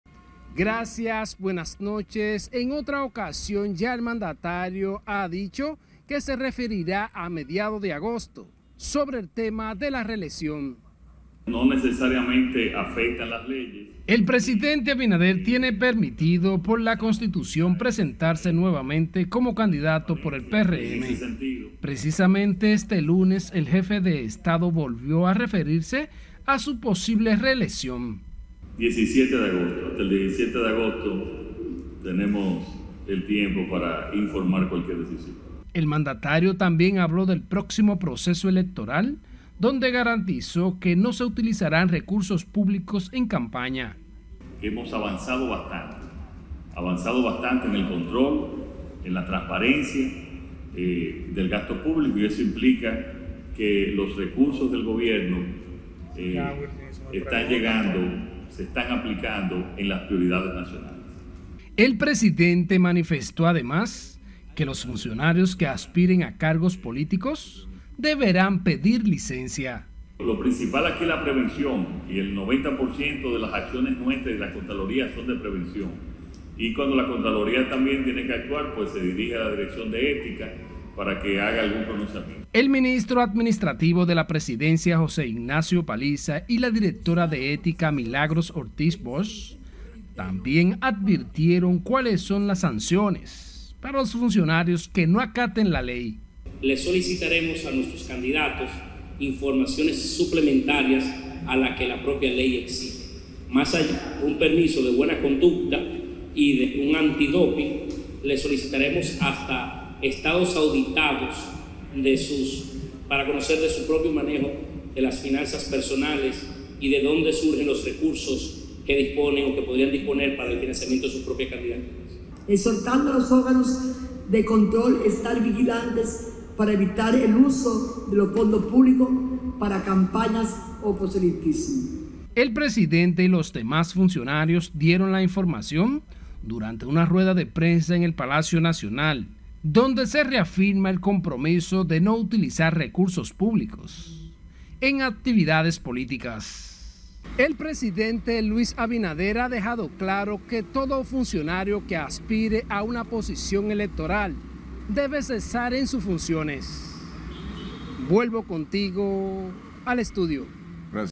Presidente Abinader asegura tiene hasta el 17 de agosto para saber si se reelige
El presidente Luis Abinader dijo este lunes que tiene hasta el 17 de agosto para decidir su repostulación para las elecciones de 2024. El anuncio fue hecho por el mandatario durante una rueda de prensa que encabezó, junto al Gabinete de Ética y Control del Gasto, donde se anunció, además, que el Gobierno garantizará el uso de los recursos públicos durante la campaña electoral.